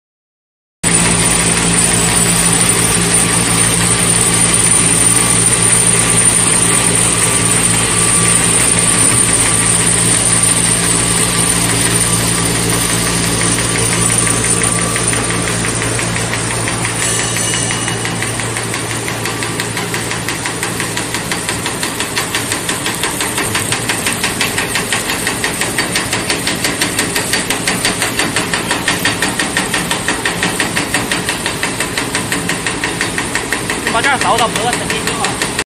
EPE Foam fruit net extrusion machine fruit net packing machinery
Jenny Wang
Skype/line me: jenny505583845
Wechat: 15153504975

Products Introduction for
EPE foam fruit and vegetable net/mesh is a new type of soft flexible packaging materials, as its unique expanding mesh structure and elastic foaming net and are widely used in production packing as glass products, precision instrument, various kinds of fruit.

Products Characteristic for

1.It can produce different kinds of fruit net ,the output is about 800,000-1,200,000per day.

2.It also can produce all kinds of foam sticks,foam pipe,foam sheet and so on by changing the mould.

3.The machine is of high output,high quality and high efficiency. at the same time we can design as required,we also sell parts of the machine.

4.Please tell me what output would you like,what kinds of foam net would you want the machine produce.I will choose right machine and send detailed information to you as soon as possible.